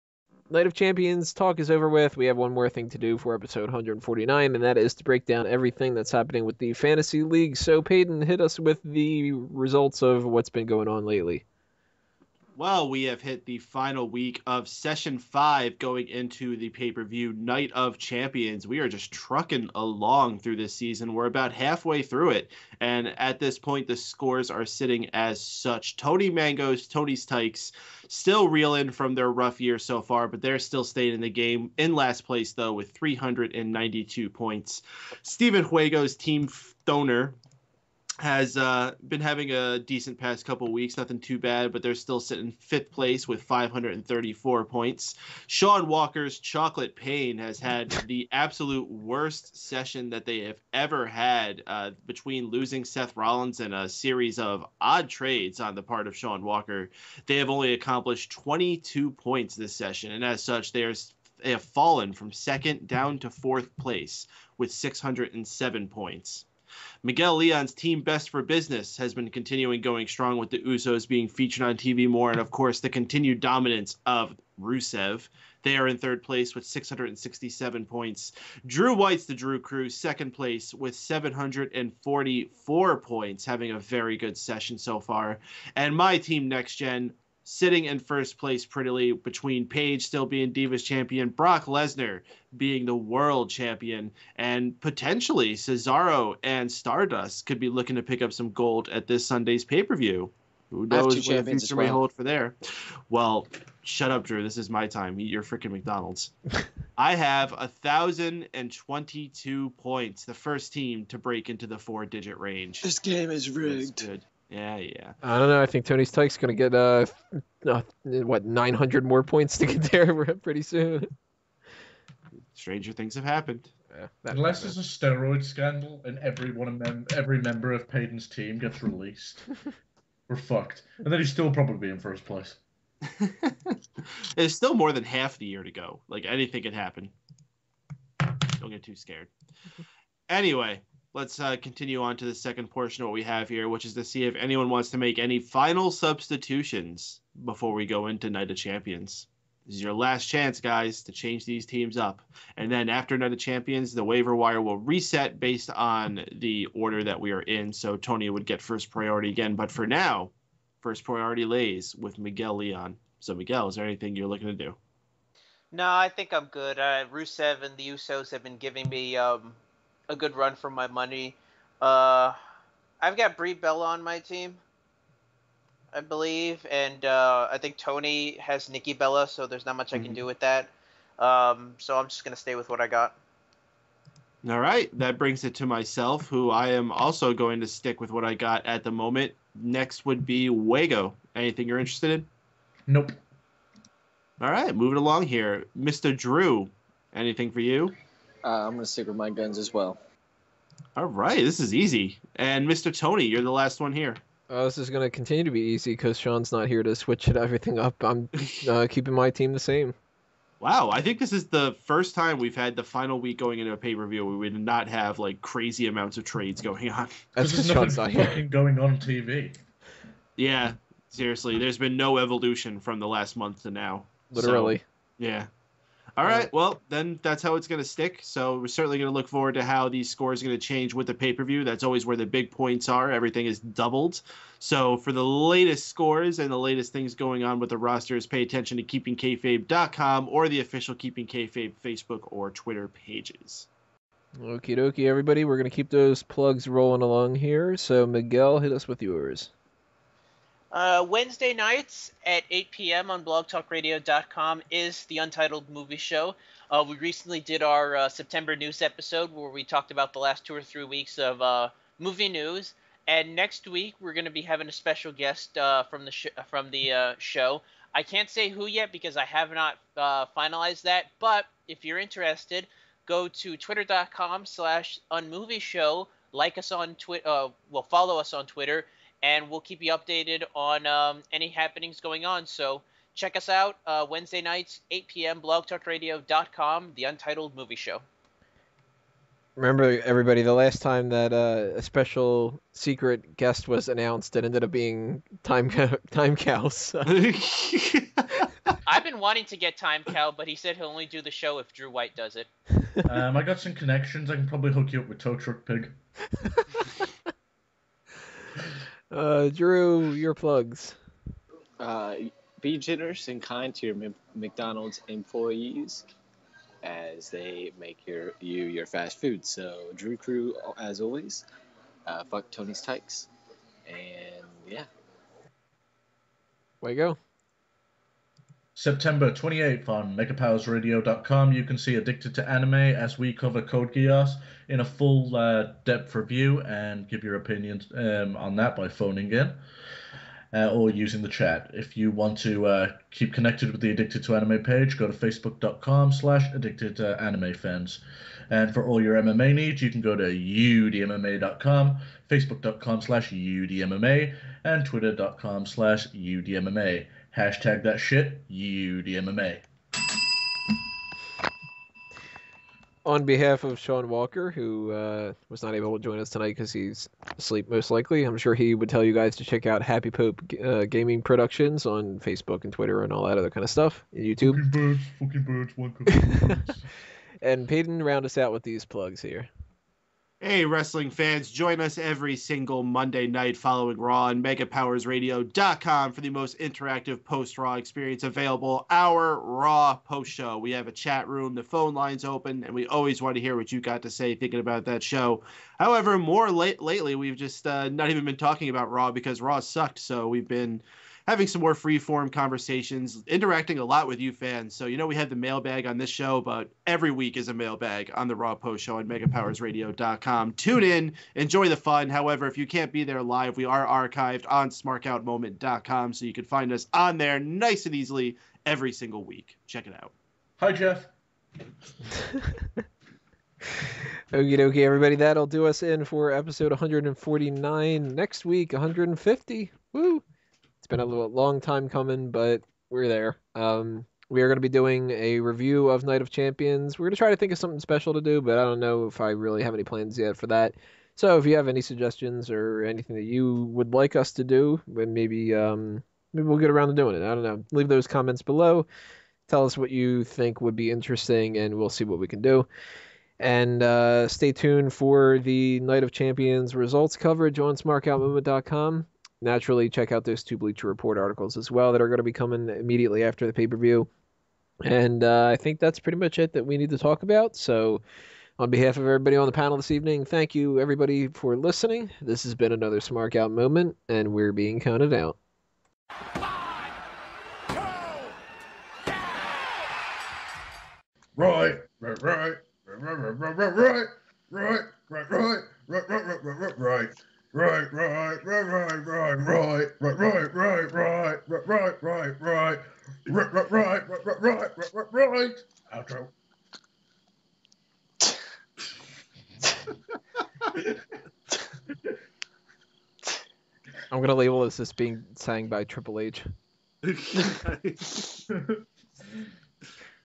ST 149 (13) WWE Fantasy League Trades and Updates
Fantasy League Trades and Updates on the Standings

Guests: @MrPaden, @SteWago, @DrewsephWhite, @UnMovieShow